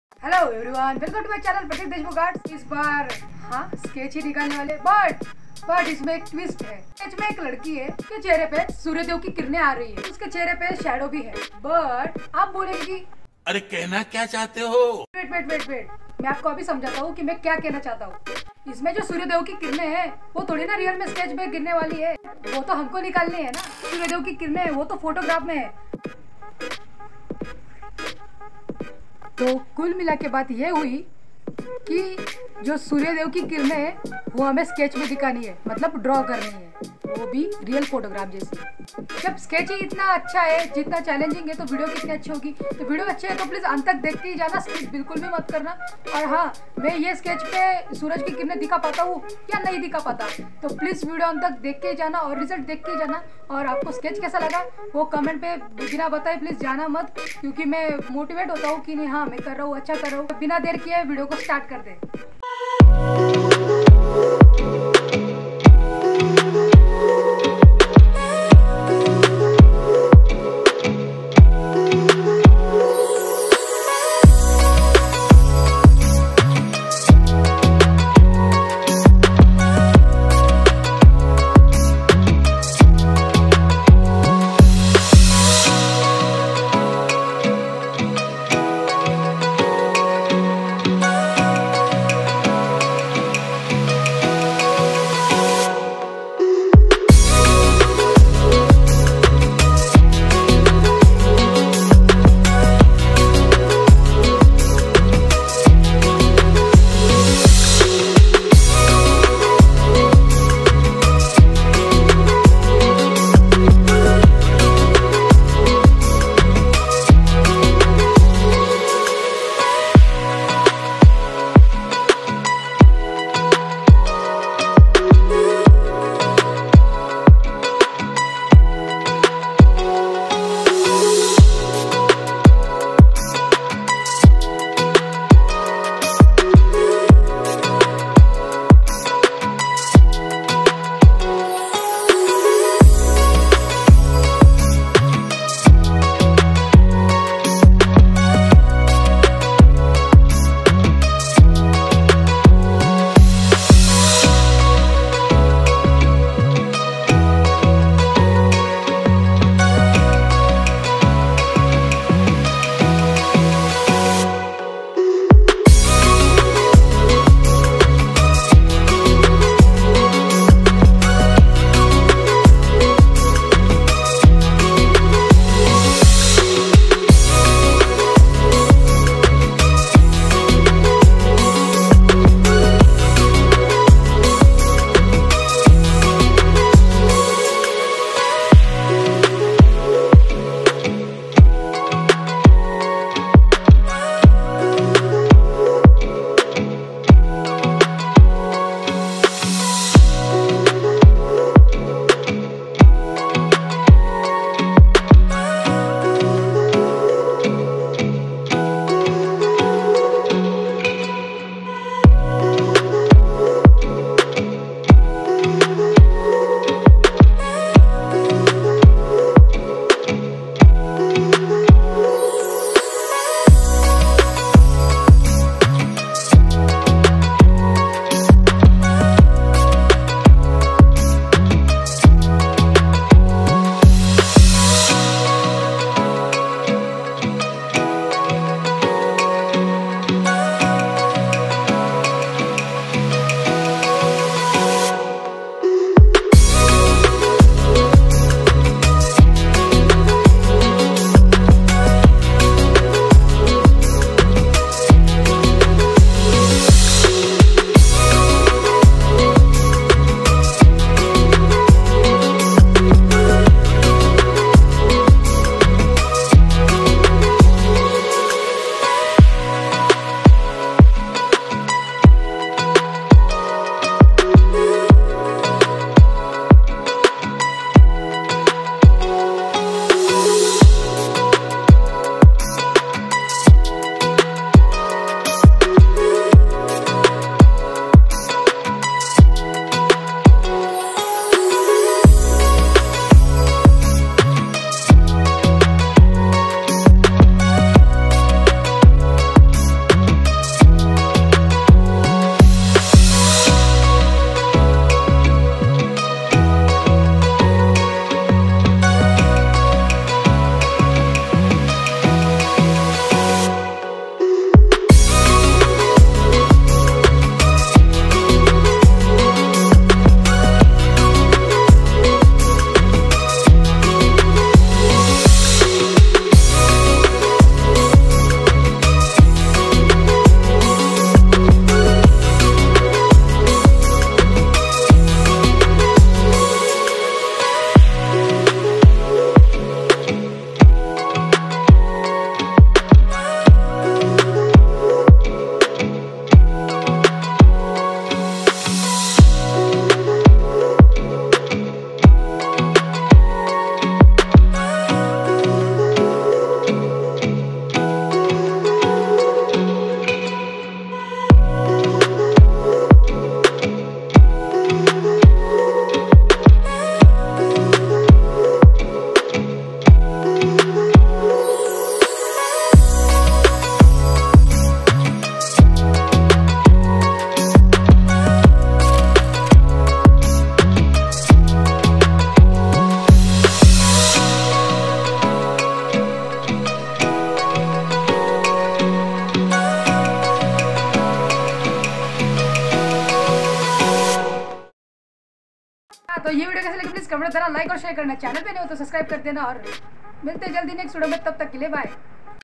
girl Realastic sketch || Pratik Deshmukh Arts
is video mein meine girl realastic nikala hai.

to

dosto is videos mein bohot mehnat hai to
please jyada se jyada share kijiye apne friends mein aur relatives mein

hyper realistic girl drawing
girl sketch
girl drawing
realistic girl
making hyperlistic drawing
colour pencil
step by step
drawing
Sketch
Pratik Deshmukh arts